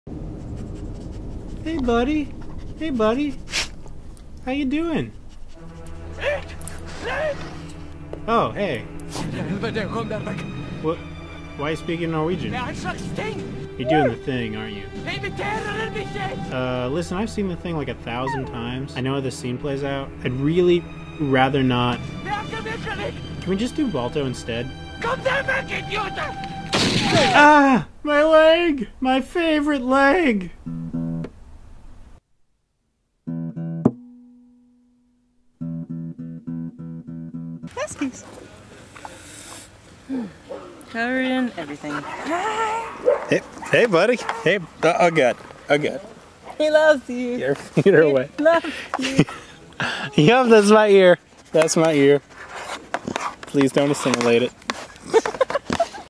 Jake's Favorite Dog
A video I shot while on vacation. This is based on one of my favorite films, "The Thing". You can help out the channel by following these links: